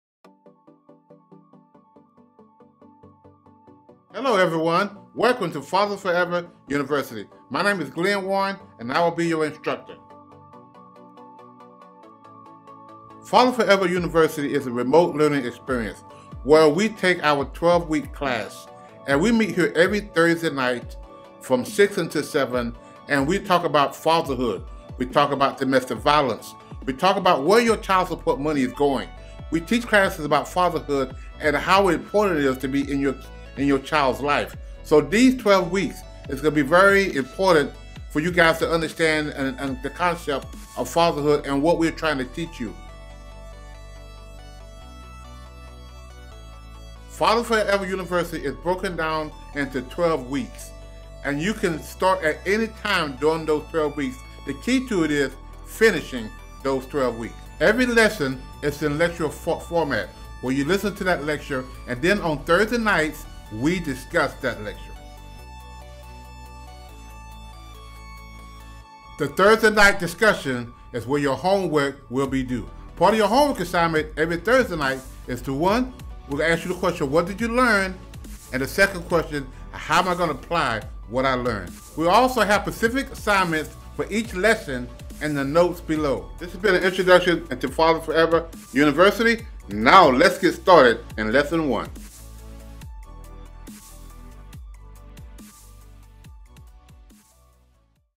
Fathers Forever University
Fathers Forever University is a 12 week remote learning experience aimed at teaching fathers the importance of their role in the family unit.